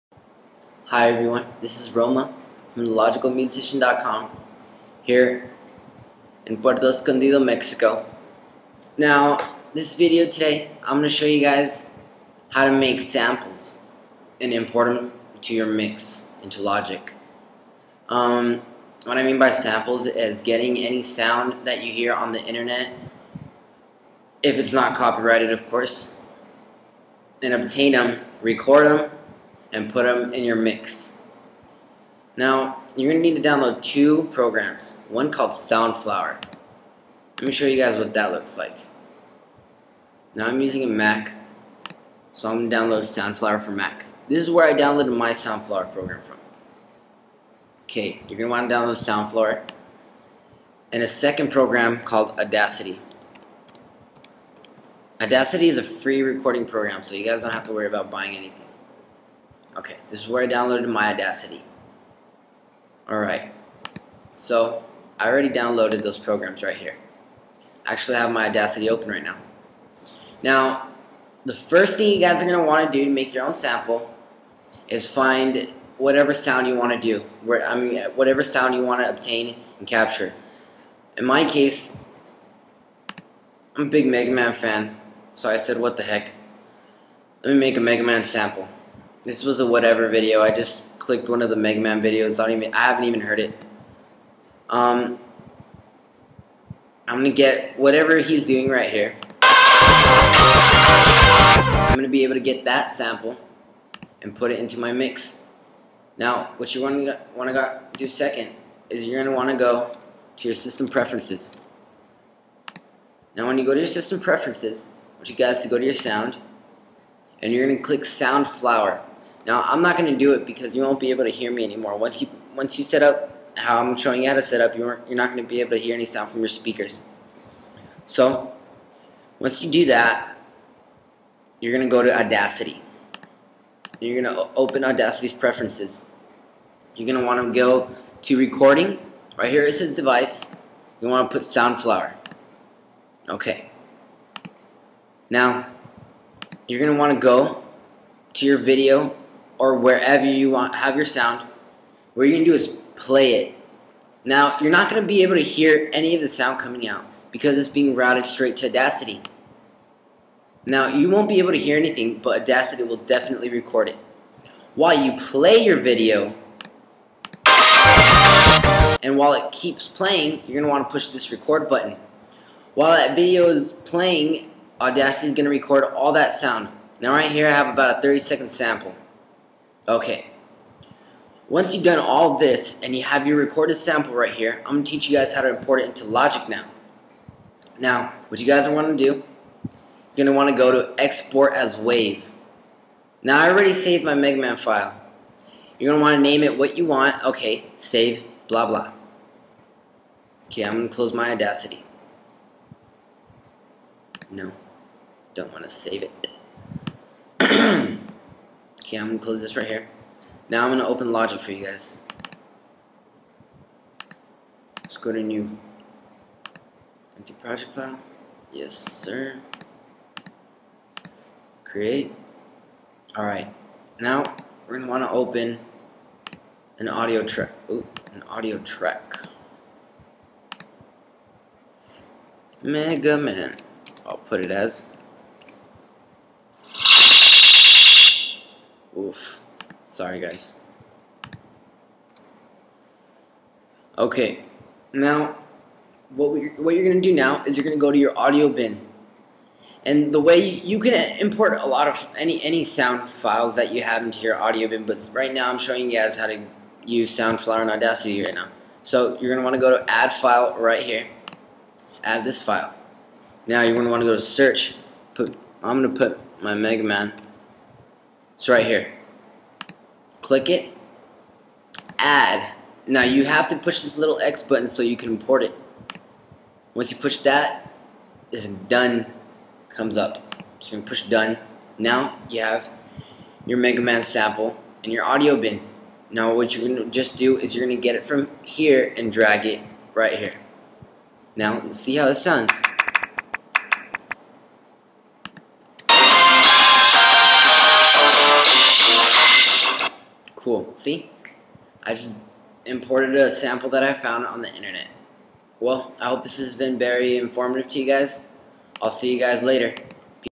How Do You Import Sound into Logic?
There 3 ways to import sound into Logic, That's by dragging and dropping from your desktop onto Logic, By adding a the file in Logic's audio bin, or in Logic's Browser. As fare as obtaining your own samples is what this video is all about.